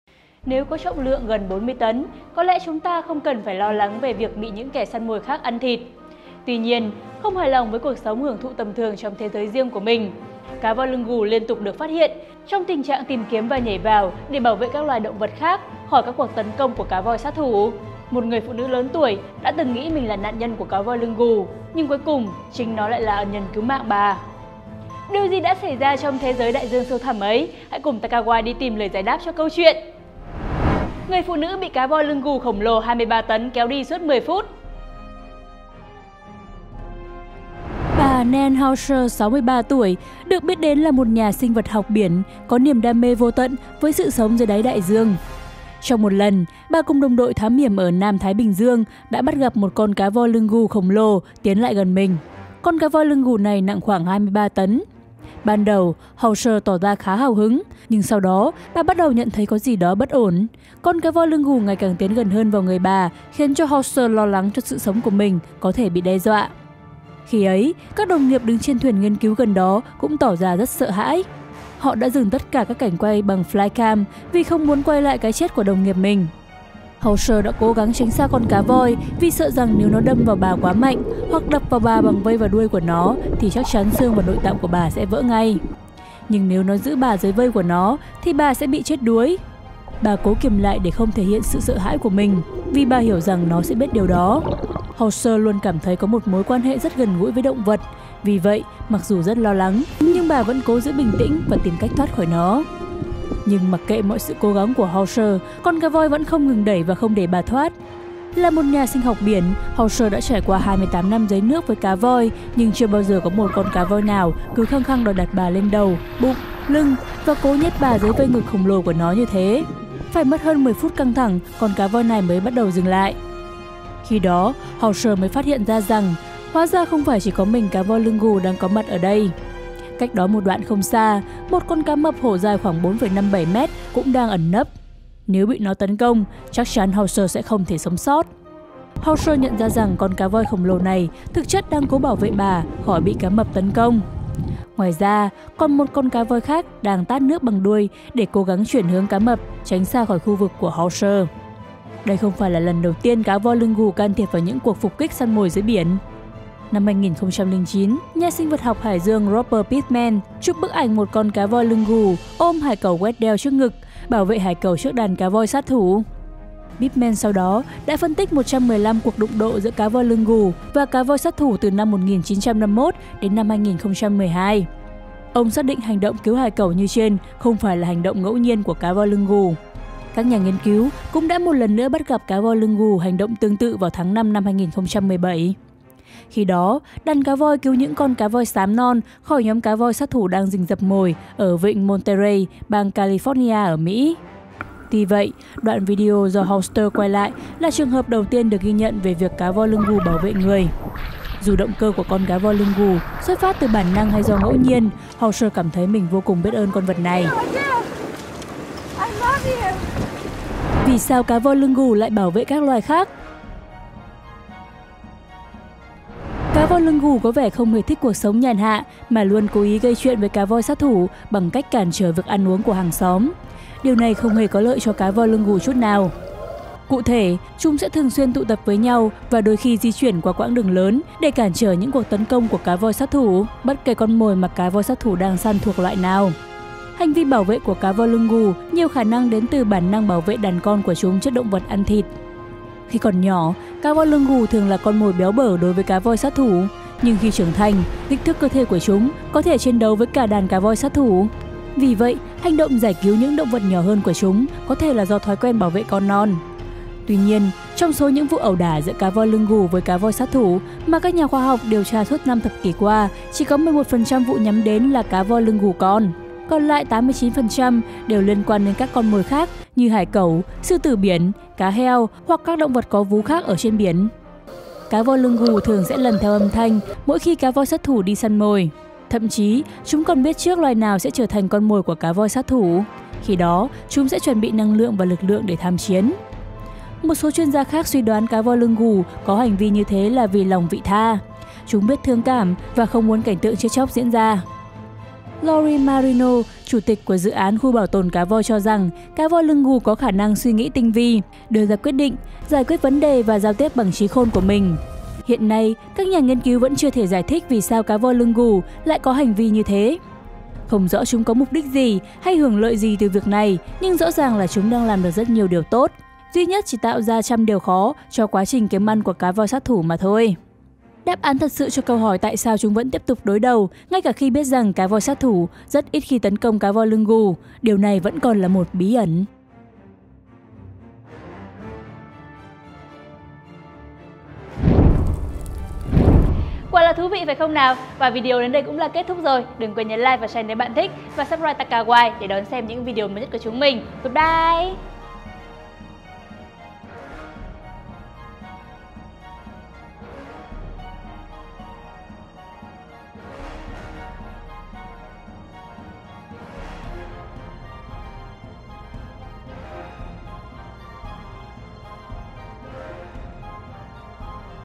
NGƯỜI THỢ LẶN NÀY BỊ CÁ VOI LƯNG GÙ KHỔNG LỒ 23 TẤN ‘KÉO ĐI’ SUỐT 10 PHÚT !?!
Còn bạn, bạn thấy video này thế nào. Để lại ý kiến của mình trong phần bình luận nhé ^^